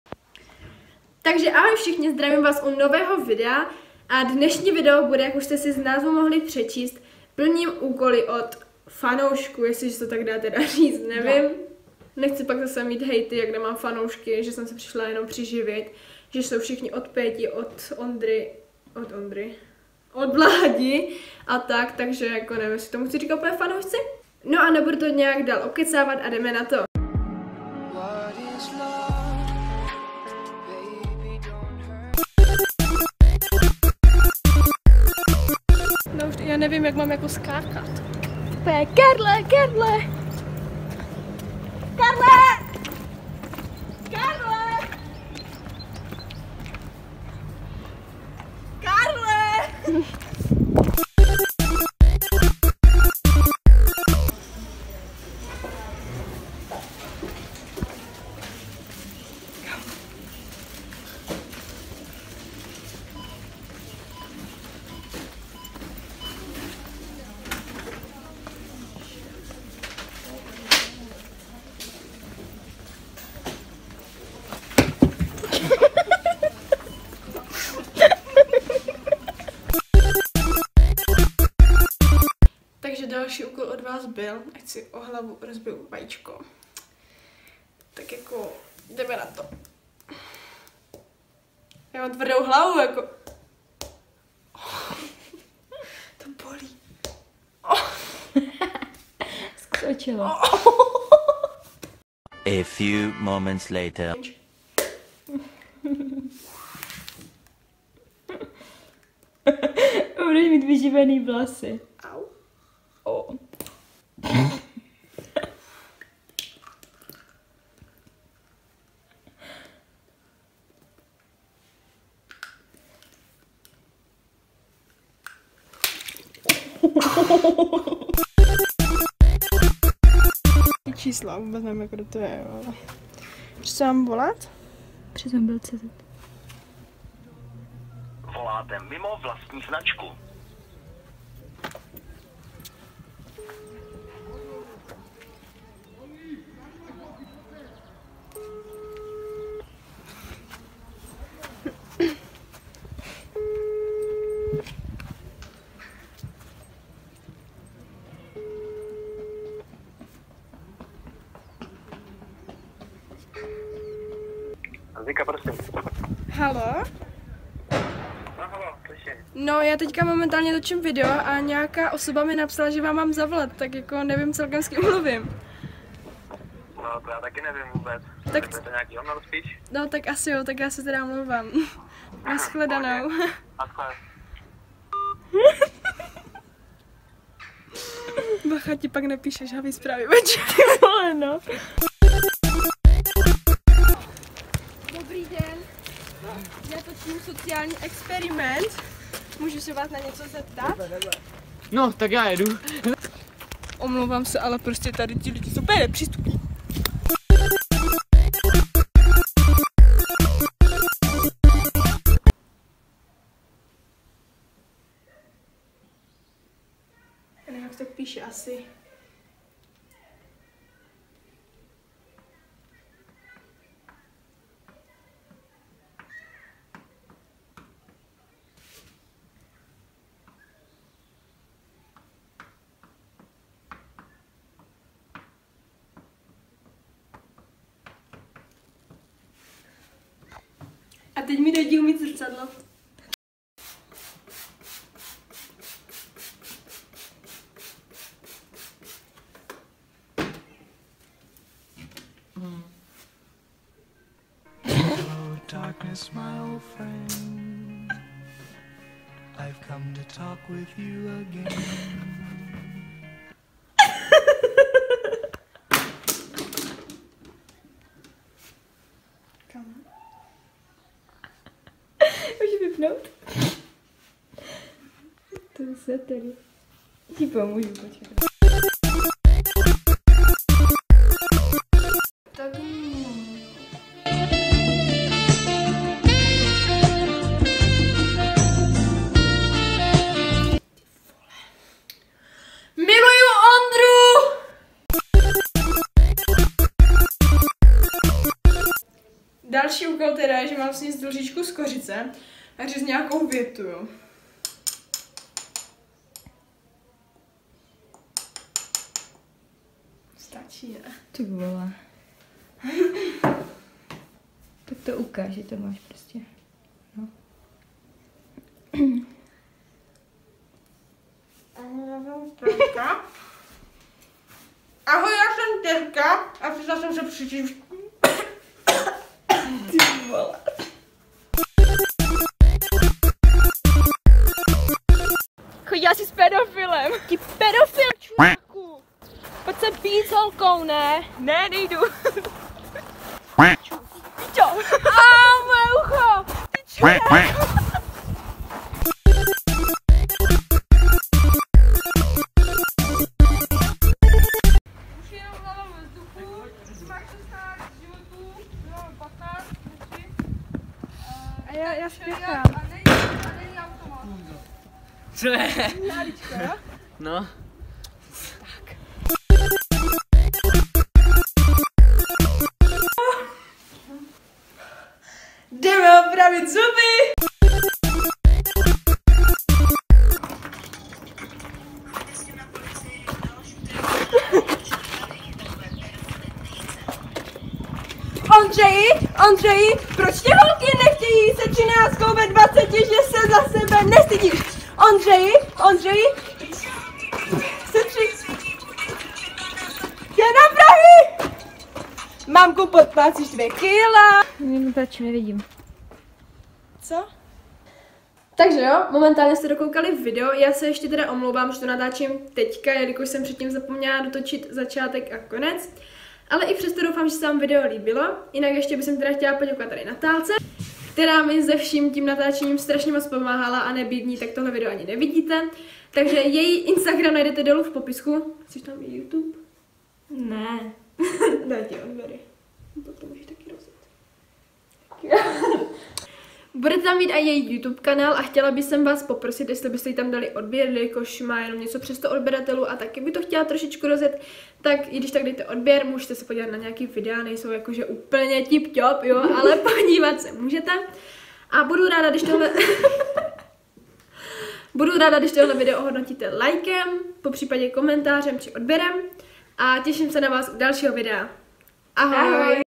OPRAVUJI ONDROVI ZUBY?🤦‍♀️PLNÍM ÚKOLY OD “FANOUŠKŮ”❤️
Ahoj, zdravím všechny co čtou popisek!😅 Děkuji moc že jste psali úkoly💖

🔐Něco málo o mně🔐
Natáčím na IPhone 7plus📱
Je mi 14 let🌝 (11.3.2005)
Jmenuji se Tereza Heinzová📝